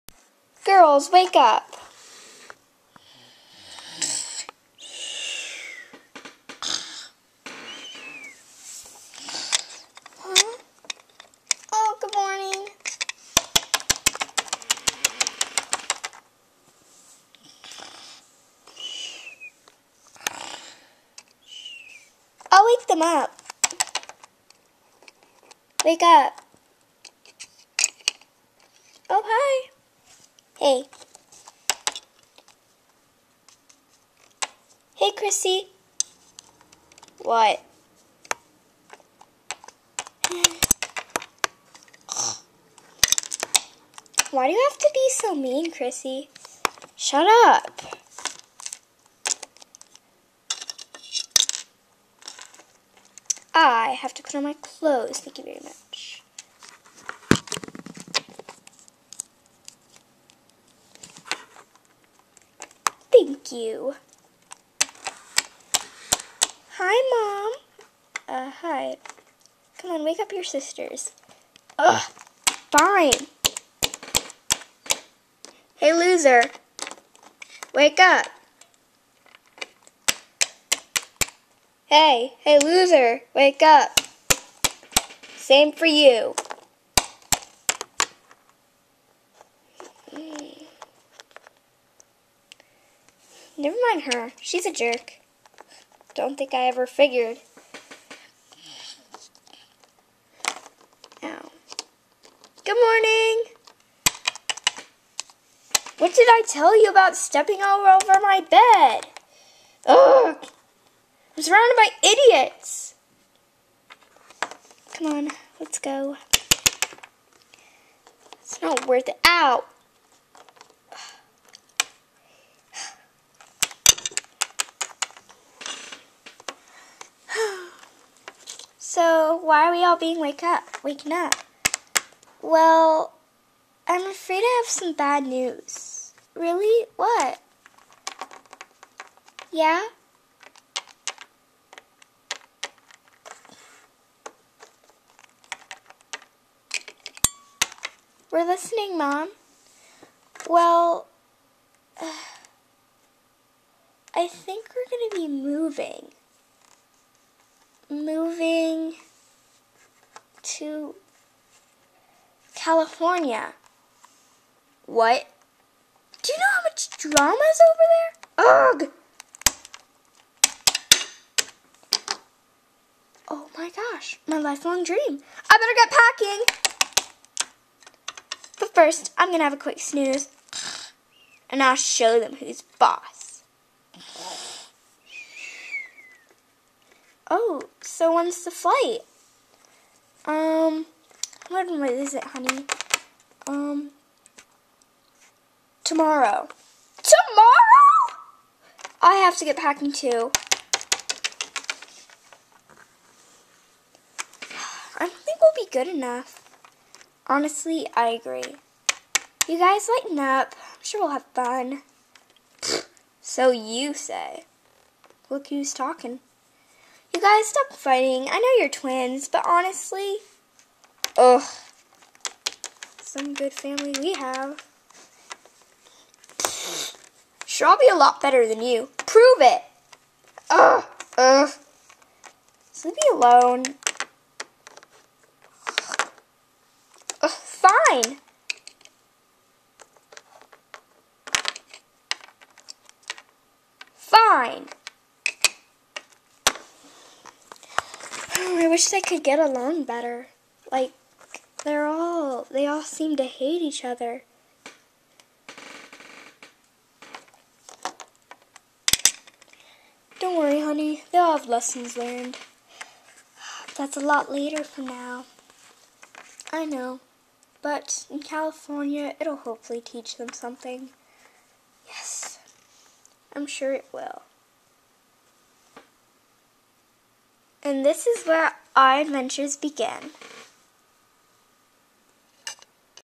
LPS: The Six Daires Of Six Kittens
So Crissy, Maddie, Lila (lie- la), Ashley, Silvia, and Goldenrod are sisters. Not the best kind since they're always fighting and hating each other. Even Silvia and Ashley are fighting, but they're twins. The only pair that get along are Silvia and Goldenrod. Will these sisters ever learn to get along? They must discover the true friendship of all and learn to trust themselfs and others.